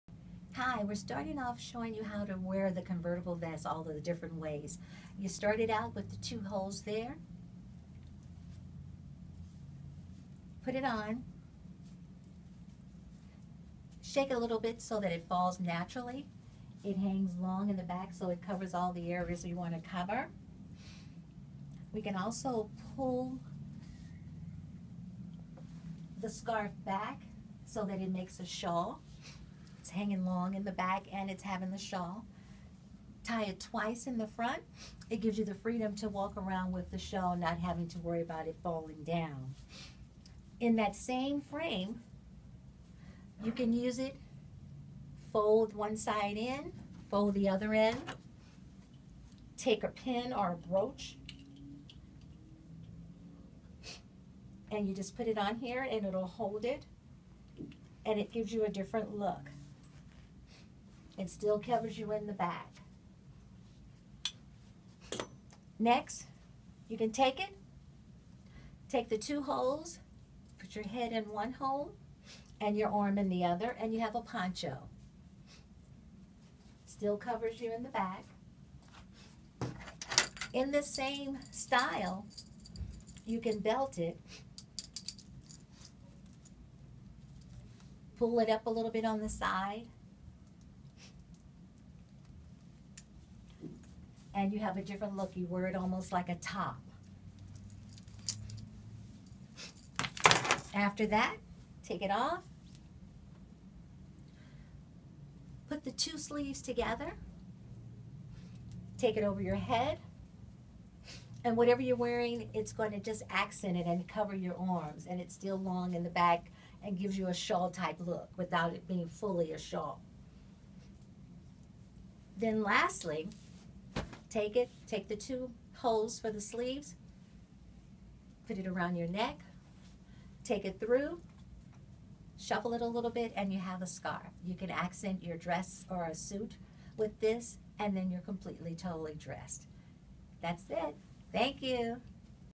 Convertible Vest Tutorial
Here are seven (7) ways to wear the ConvertibleVest from in Style Boutique.